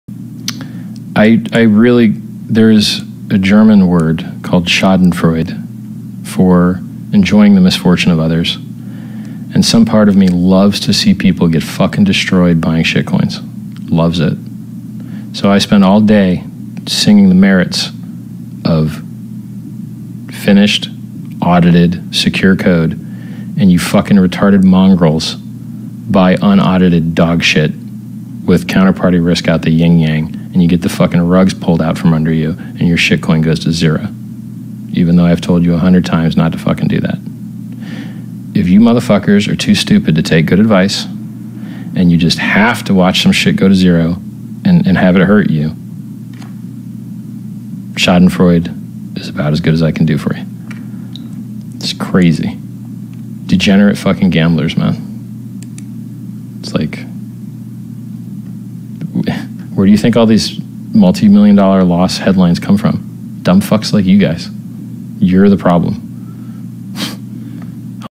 HEX Is Audited Three Times And Is Complete Schádenfreude
Schádenfreude motherfuckers.

Stop investing in shit projects that are not complete and that promise you the world (Those are called securities). HEX is complete and audited Three Times!

Other Places You Can Find Me:

Dlive.tv/ballietbran (Please follow me on Dlive I need 200 followers until I can host!!!)

Twitter.com/ballietbran

Twitter.com/ballietbran